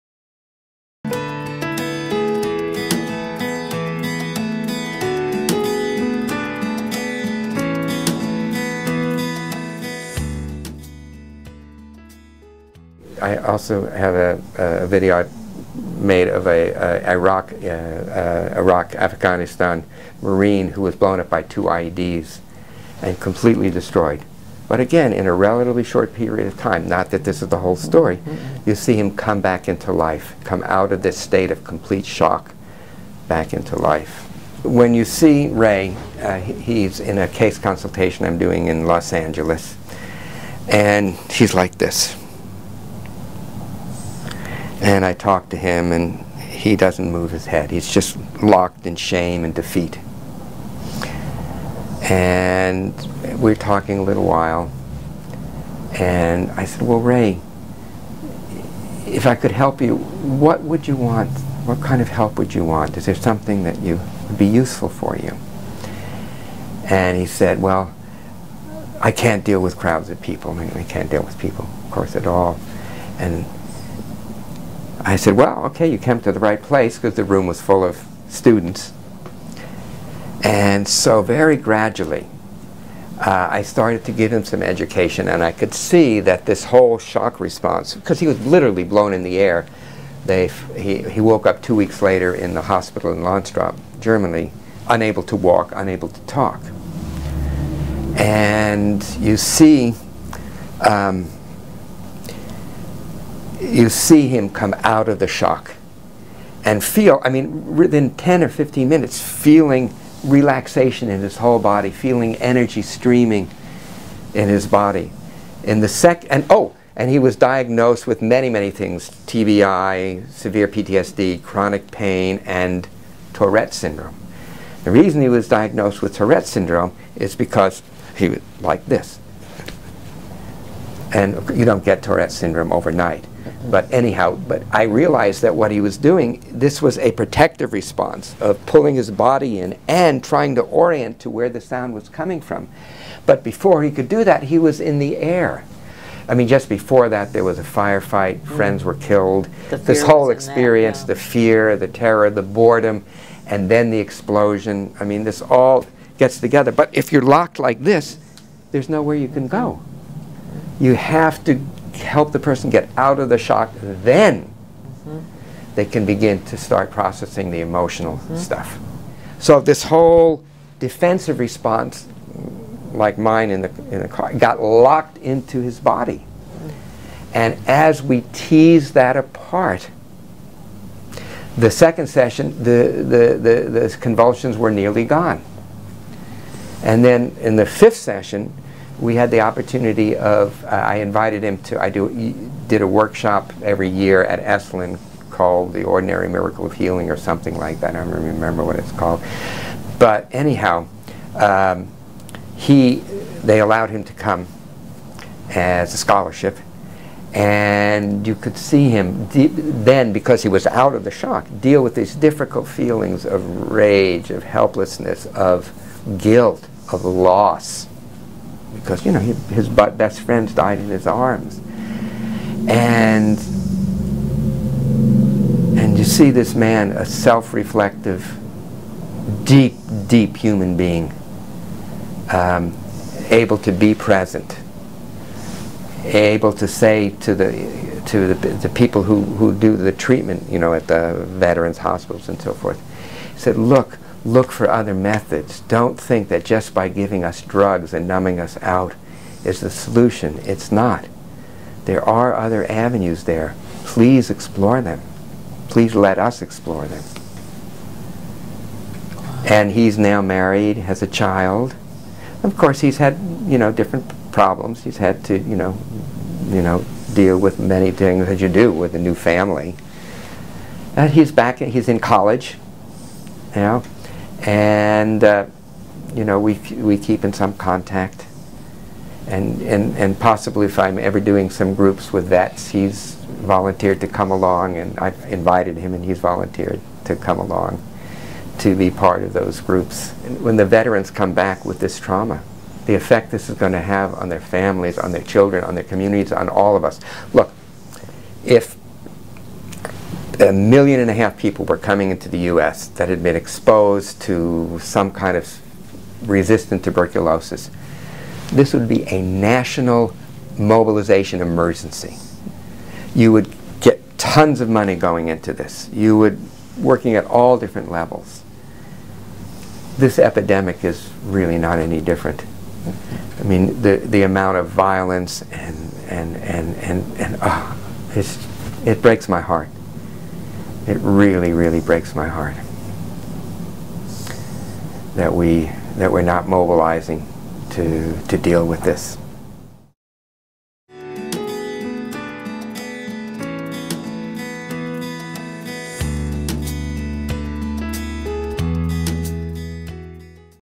Dr. Peter Levine on the Somatic Experiencing Approach and PTSD
Dr. Peter Levine describes a case example of the Somatic Experiencing Approach for Combat Post Traumatic Stress Disorder.